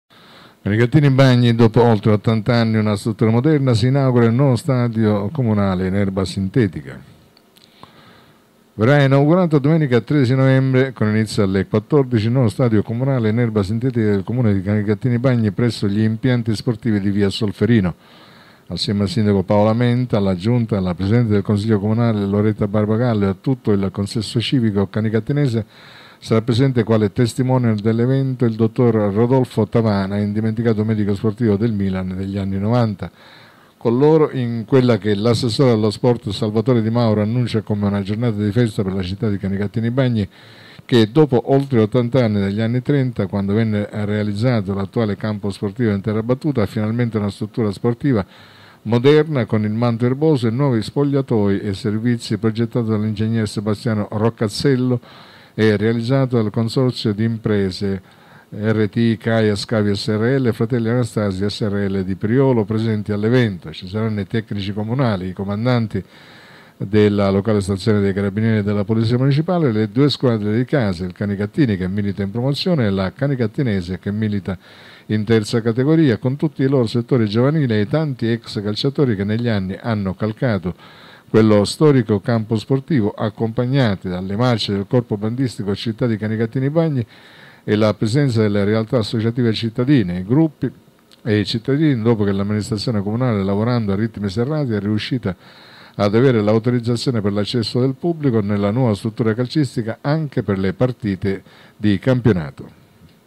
Canicattini Bagni Dopo oltre 80 anni, una struttura moderna «Si inaugura il nuovo Stadio comunale i
Verrà inaugurato domenica 13 novembre 2022 con inizio alle ore 14 il nuovo Stadio comunale in erba sintetica del Comune di Canicattini Bagni presso gli impianti sportivi di via Solferino.

Assieme al Sindaco Paolo Amenta, alla Giunta, alla Presidente del Consiglio comunale Loretta Barbagallo e a tutto il Consesso civico canicattinese, sarà presente, quale testimonial dell’evento, il Dott. Rodolfo Tavana, indimenticato Medico sportivo del Milan degli anni ’90.

Con loro, in quella che l’Assessore allo Sport Salvatore Di Mauro annuncia come una giornata di festa per la città di Canicattini Bagni che dopo oltre 80 anni, dagli anni ’30, quando venne realizzato l’attuale campo sportivo in terra battuta, ha finalmente una struttura sportiva moderna con il manto erboso e nuovi spogliatoi e servizi progettata dall’Ing. Sebastiano Roccazzello e realizzata dal consorzio d’imprese R.T.I. Kaya Scavi srl e F.lli Anastasi srl di Priolo Gargallo, presenti all’evento, ci saranno i tecnici comunali, i Comandanti della locale Stazione dei Carabinieri e della Polizia Municipale, le due squadre di casa, il Canicattini che milita in Promozione, e la Canicattinese che milita in Terza Categoria, con tutti i loro settori giovanili, e i tanti ex calciatori che negli anni hanno calcato quello storico Campo sportivo, accompagnati dalle marce del Corpo Bandistico Città di Canicattini Bagni, e la presenza delle realtà associative cittadine, i gruppi e i cittadini, dopo che l’Amministrazione comunale, lavorando a ritmi serrati, è riuscita ad avere l’autorizzazione per l’accesso del pubblico nella nuova struttura calcistica, anche per le partire di Campionato.

La manifestazione, presentata da Principe Giank (Giancarlo Cultrera), dopo il taglio del nastro alla presenza dei parroci Don Sebastiano Ferla e Don Maurizio Casella che benediranno la nuova struttura, prevede l’intrattenimento da parte dei ragazzi dei settori giovanili delle due formazioni calcistiche cittadine, l’esibizione del Corpo Bandistico Città di Canicattini Bagni e una partitella conclusiva a ranchi misti tra Canicattini e Canicattinese.

Inoltre, in mattinata in Piazza XX Settembre, a cura dell’ASAS, Associazione Siracusana Auto-motoveicoli Storici presieduta da Diego Morreale, dalle ore 10 alle 13 si svolgerà il raduno d’auto d’epoca.

Il progetto del nuovo Stadio comunale in erba sintetica nasce nel 2017 con l’attuale Sindaco Paolo Amenta e l’Assessore allo Sport Salvatore La Rosa che ne avviarono tutte le procedure per il finanziamento (oltre un milione di euro) attraverso i fondi del Programma FSC 2014/2020 “Patto per la Sicilia” da parte dell’Assessorato regionale del Turismo, dello Sport e dello Spettacolo, iter concluso in piena continuità amministrativa e programmatica in tutte le fasi di gara e realizzazione dal Sindaco Marilena Miceli, oggi presente in Giunta col Sindaco Paolo Amenta.